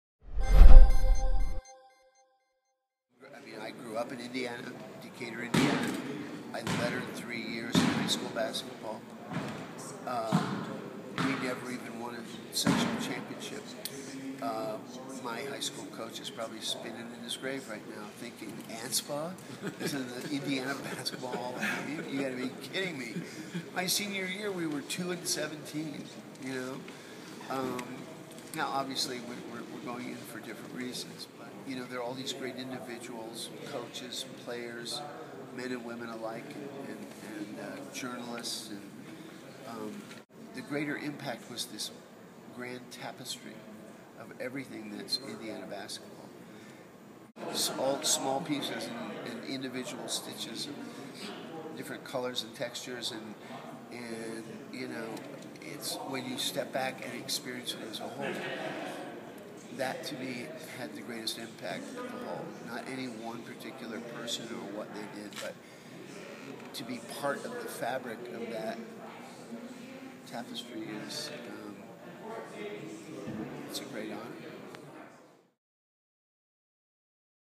Hoosiers director David Anspaugh: "My high school coach is probably spinning in his grave."
Anspaugh is being inducted into the Indiana Basketball Hall of Fame.